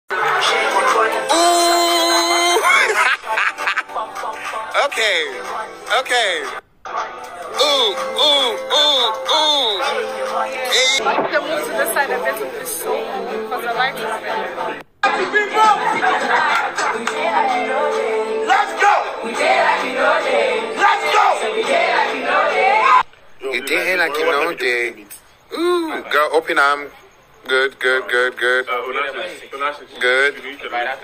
Sister Deborah - Shooting A New Music Video
Sister Deborah, known as Sister Derby was spotted shooting a new music video. This is sure going to cause stir in the internet.

Sister deborah
ghana
sister derby
deborah vanessa
ghanaweb tv